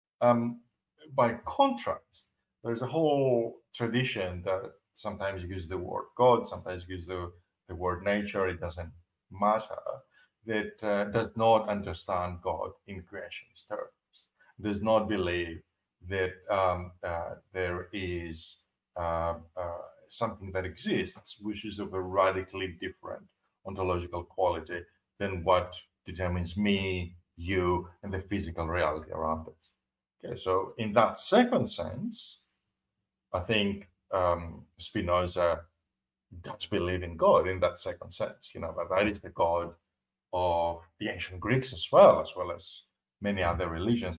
Spinoza's God - The God of the Ancient Greeks
Excerpted from my podcast with Professor Dimitris Vardoulakis, he discusses the way in which Spinoza believes in God/nature; something scholars have been arguing what he meant for centuries and still don't agree.